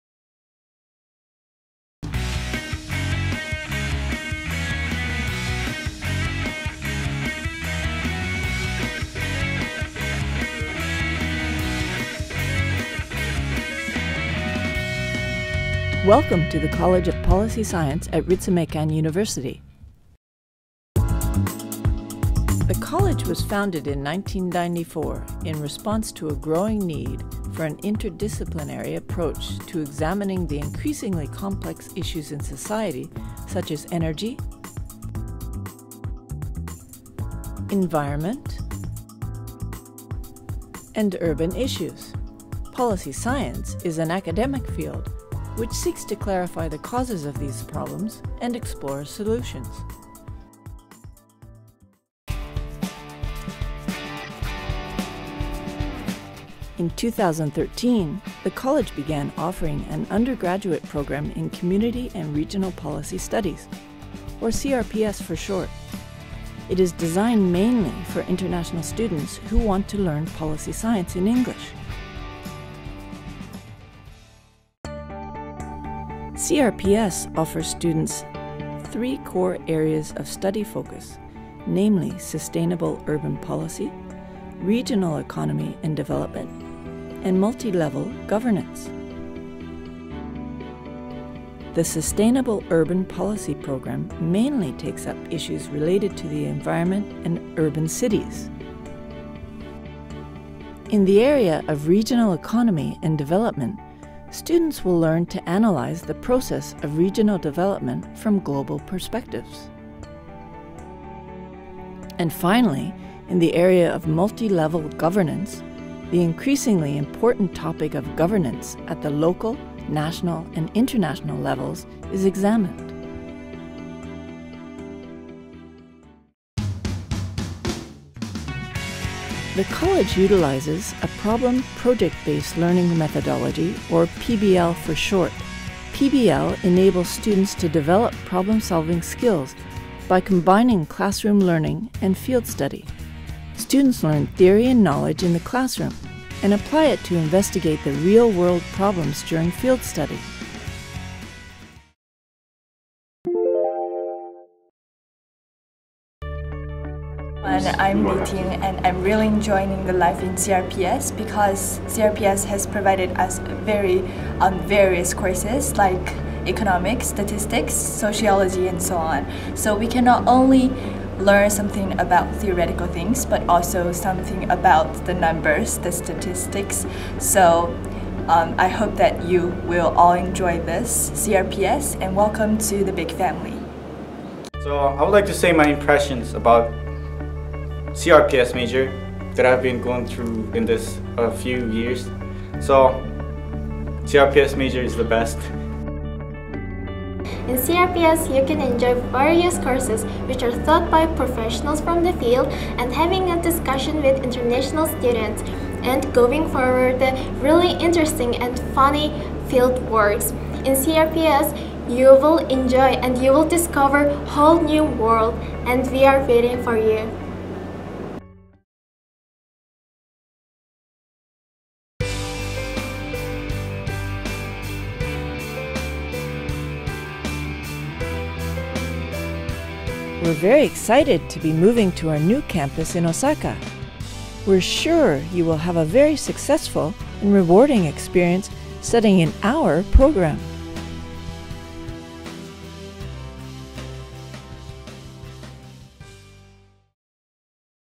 ［Ritsumeikan University］College of Policy Science, Community and Regional Policy Studies Major
Introduction video of Community and Regional
Policy Studies Major in the College of Policy Science, Ritsumeikan
University.